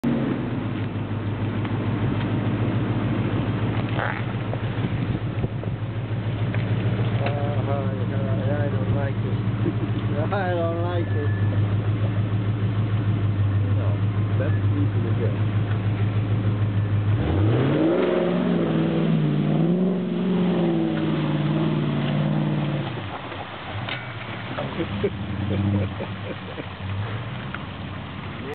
Land Rover Upper Peninsula Creek Crossing
Crossing a creek in the UP, near Mt. Arvon.